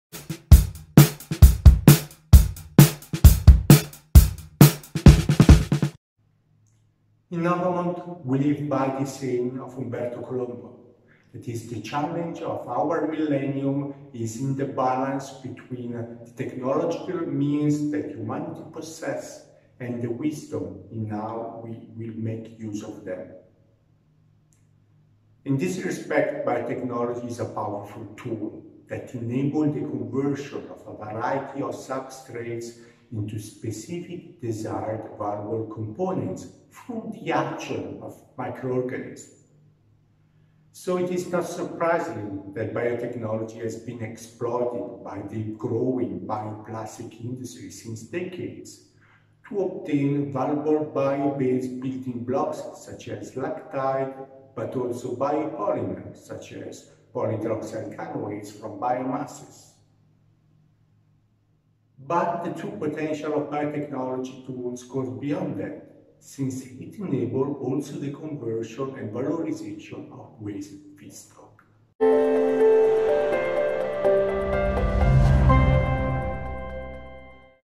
European Biotech Week - Episode 01
From September 27 to October 3 2021, Novamont joins the European Biotech Week, an occasion to celebrate science, innovation and biotechnology. Discover more by following Novamont's webseries "International Bioeconomy and Biotechnology".